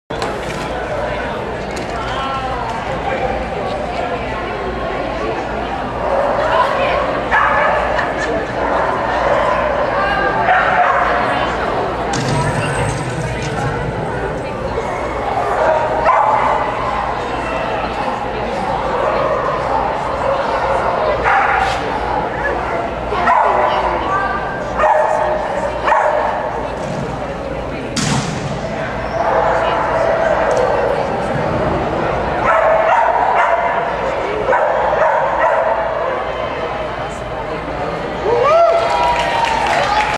Token & Beth Diehl, ExB Std Crystal Lake, IL 7/15/06
Interesting choice to rear cross after the triple. It also looks like she planned to front cross after the tire but  there was no time, so she did a nice rear cross at the teeter instead.